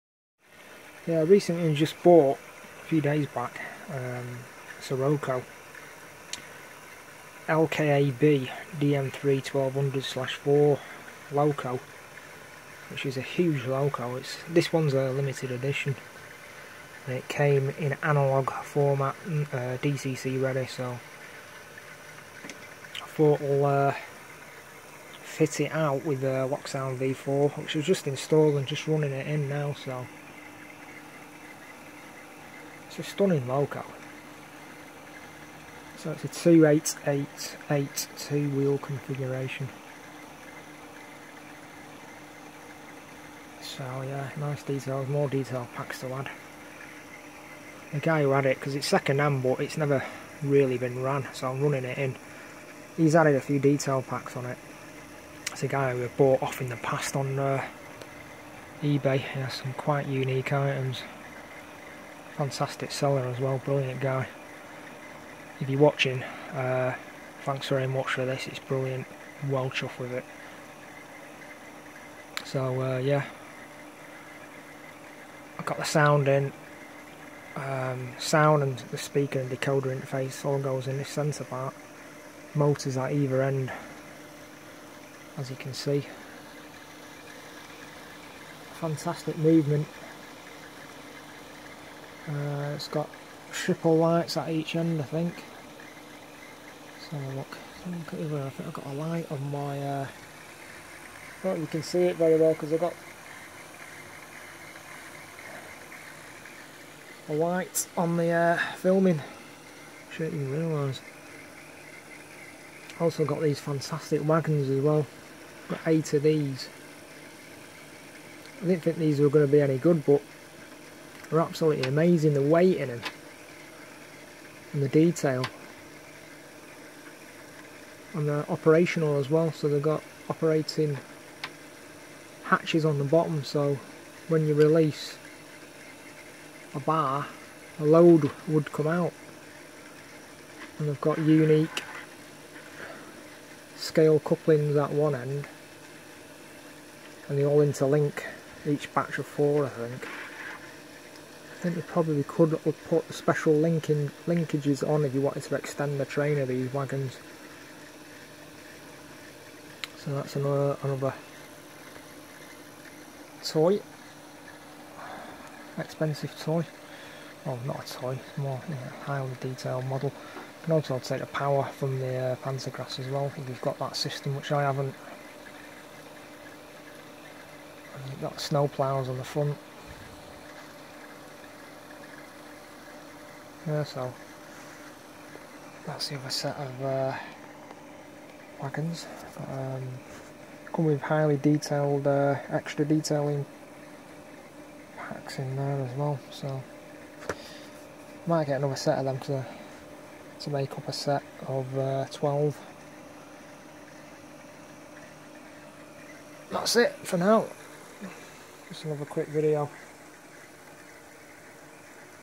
ROCO 63754 CLASS DM3 1200 (year 2005)
The iron ore trains on the Iron Ore Line between Luleå in Sweden and Narvik in Norway were originally operated by the Of class locomotives, based on the earlier O-series locomotives first used on the line from 1914. In order to replace these locomotives, SJ chose to order new locomotives for the Iron Ore Railway based on the new Da class locomotives being ordered at the time. An initial twenty-four twin-section locomotives, designated the Dm class, were ordered and delivered in 1953, with a further fifteen fitted with new, more powerful motors, delivered in the 1960s. As a result of this, four locomotives from the first batch were fitted with the more powerful motors to match.
As initially developed, the twin-section Dm class locomotives were capable of pulling 3,400 tonne trains. However, due to progressive increases in train size and weight this was no longer acceptable by the end of the 1960s, and so SJ ordered 19 new cabless centre locomotives, which were installed permanently between the two halves of a Dm class locomotive; the resulting rebuild being classed as Dm3. Due to the addition of the centre locomotive, the Dm3 class locomotives were able to haul trains up to 5,400 tonnes. Around the same time, the locomotive cabs were modernised with new, rationalised control layouts and additional noise insulation. Rather than copy its neighbour, the Norwegian State Railway chose to build a different and more powerful locomotive, the NSB El 15 to modernise its operations around the same time.
In the 1980s, the majority of the remaining Dm class locomotives were moved to other locations on the SJ network, leaving the Dm3 class locomotives to operate the Iron Ore Line. All of the Dm3 and several Dm class units passed with the Iron Ore Line to the private company MTAB in 1996, with the remaining Dm class units being reclassified Dm2. With further increases in train size, MTAB decided to order new locomotives to replace the older Dm2 and Dm3 units. With the arrival of the first new IORE locomotives in 2000, the entire Dm2 class were removed from service along with several Dm3 class locomotives. In 2004 one set was donated to the heritage society Malmbanans Vänner, who also own one of the similar-design Norwegian El 12 class locomotives as used on the Iron Ore Railway. MTAB have since agreed that the remaining Dm3 class locomotives will be retired in 2011, when enough IORE locomotives are available to relieve them.

Model: In blue-grey livery. Wheel arrangement 2-8+8+8-2. Permanently coupled triple locomotive with close-coupling mechanism between the three parts and at the ends. Body plastic, frame die-cast metal, the two outer locomotive parts are driven by one motor each. 2 x 2 flywheels, drive on eight axles, eight wheels with traction tyres.

· Unique movement of the three side rod systems

· Two motors

· Close-coupled locomotive sections

· DC version optionally also available with load-regulated digital decoder and original locomotive sound